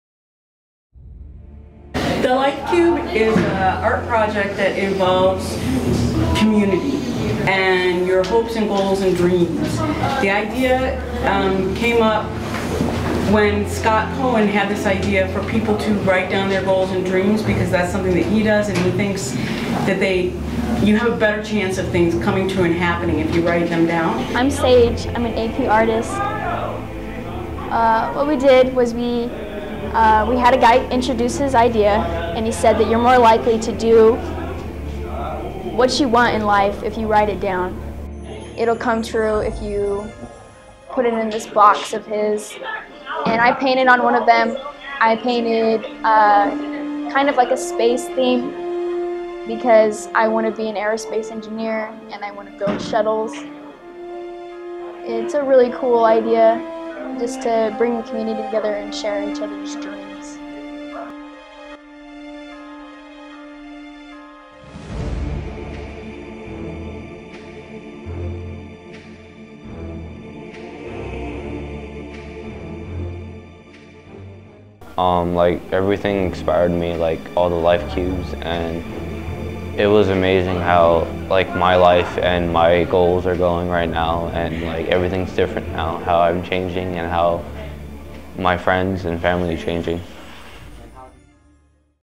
Art news feature by Daniel Rivera 2016
Students from our high school were able to be apart of the Life Cube Project in downtown Las Vegas. Students created artistic pieces that were added to a huge cube that embodied their life's goals and dreams to then be burt and shared with the world.